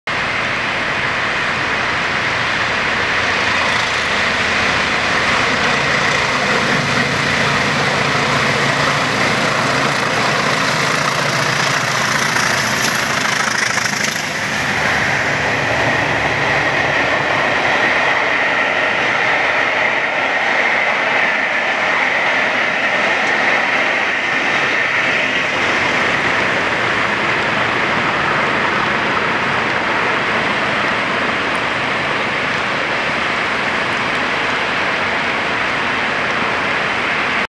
20305 + 20308 thrash passed Darlaston Junction on 'The Darley Dimple & Dale'
20305 + 20308 thrash passed Darlaston Junction on 1Z67 Matlock Peak Rail to Crewe, return charter 'The Darley Dimple & Dale'. 12/04/15